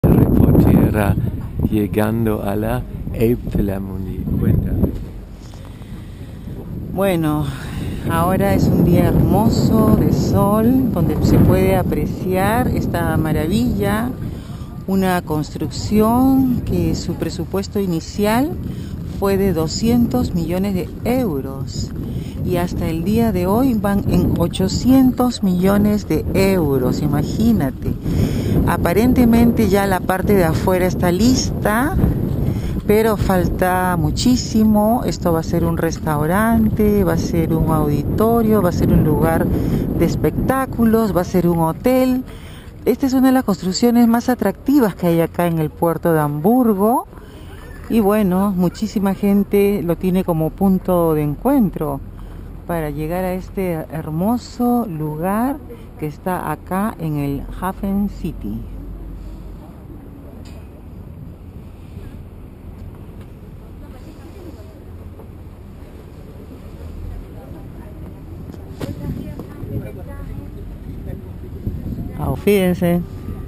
Elbe Philarmonic una construcción de 800 millones de Euros en el Puerto de Hamburgo
Corrección el Presupuesto inicial era 70 millones de Euros